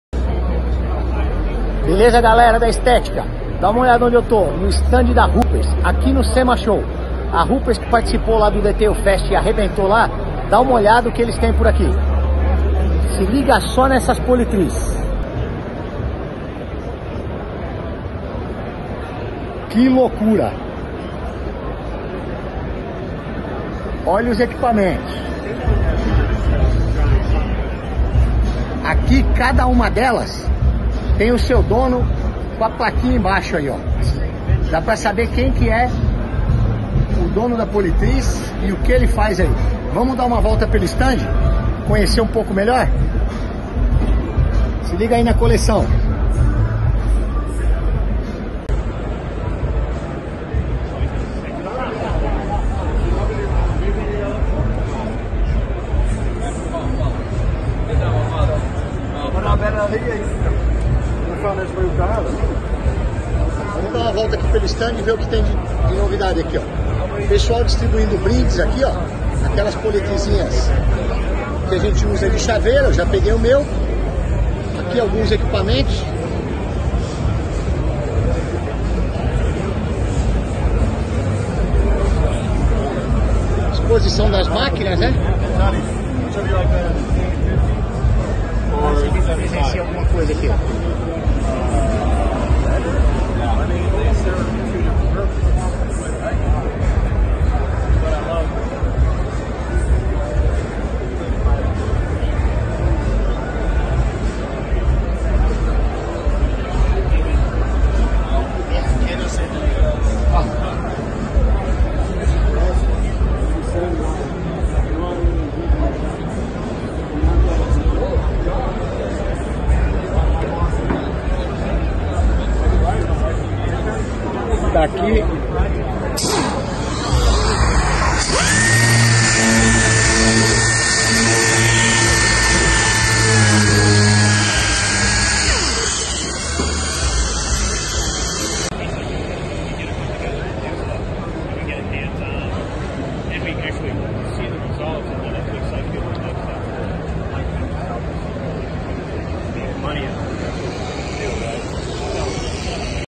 Conheça o Stand da Rupes no Sema Show em Las Vegas - Rgolden Cursos Automotivos
Estamos no Sema Show aproveitamos para mostrar para vocês o Stand da Rupes

Para mais informações sobre Cursos Automotivos ou Ferramentas Entre em contato.